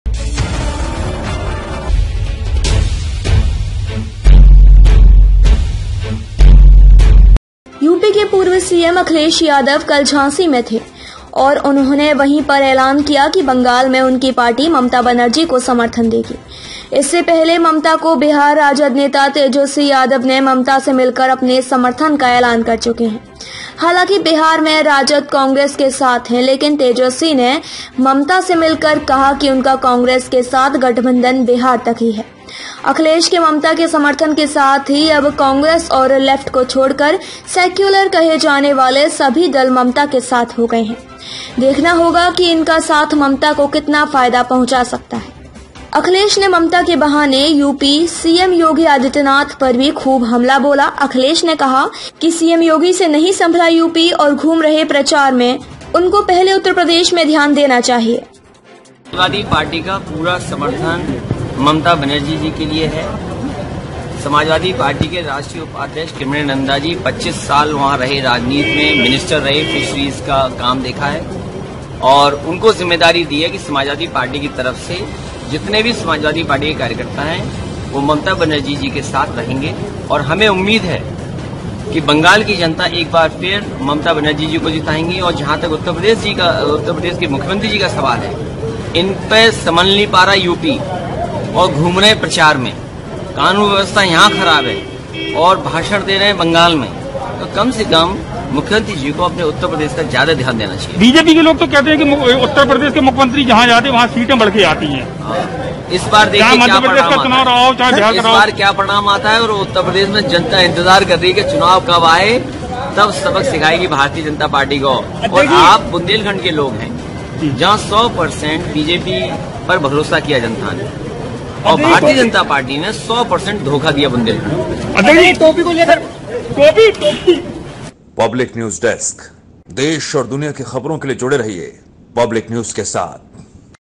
ममता को अखिलेश का भी मिला साथ पर क्या बनेगी बात publicnewstv
यूपी के पूर्व सीएम अखिलेश यादव कल झाँसी में थे और उन्होंने वहीं पर ऐलान किया कि बंगाल में उनकी पार्टी ममता बनर्जी को समर्थन देगी। इससे पहले ममता को बिहार राजद नेता तेजस्वी यादव ने ममता से मिलकर अपने समर्थन का ऐलान कर चुके हैं। हालाँकि बिहार में राजद  कांग्रेस के साथ है लेकिन तेजस्वी ने ममता से मिलकर कहा कि उनका कांग्रेस के साथ गठबंधन बिहार तक ही है। अखिलेश के ममता के समर्थन के साथ ही अब कांग्रेस और लेफ़्ट को छोड़कर सेल्यूलर कहे जाने वाले सभी दल ममता के साथ हो गये हैं। देखना होगा कि इनका साथ ममता को कितना फ़ायदा पहुँचा सकता है।